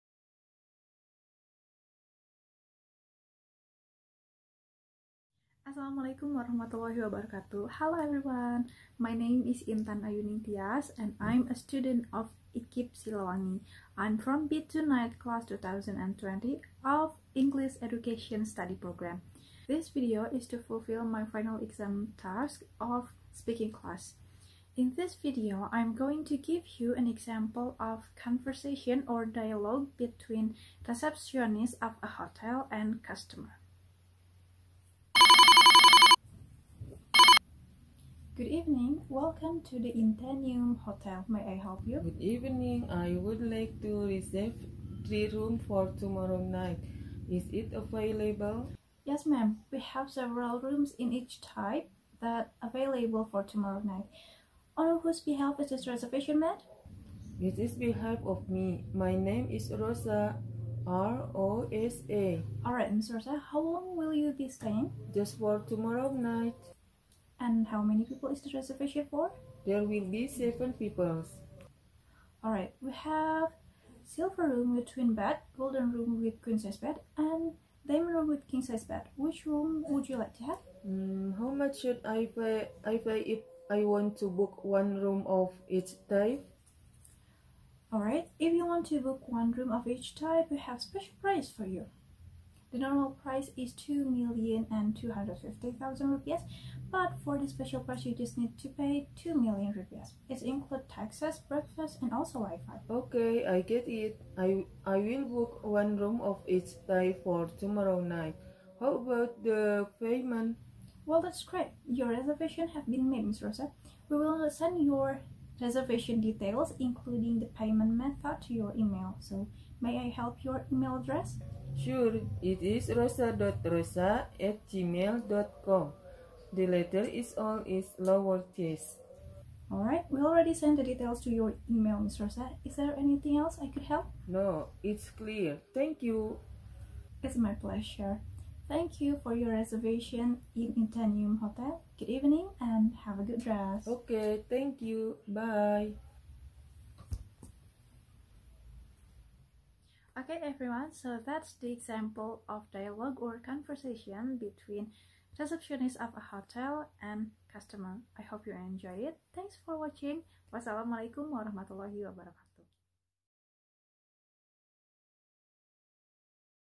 EXAMPLE DIALOGUE : Receptionist of a Hotel and Customer
Assalamu'alaikum, Hi Guys 😊👋

I present you the best of the best Hotel's Receptionist ever and her customer, when they had a conversation by phone in a beautiful night... 🤣🤣🤣🤭

Actually I made this video to fulfill my final exam task of speaking class in college. Hope it helpful, and entertaining (maybe hahaha 🤣), happy watching...

Enjooooooy 🥳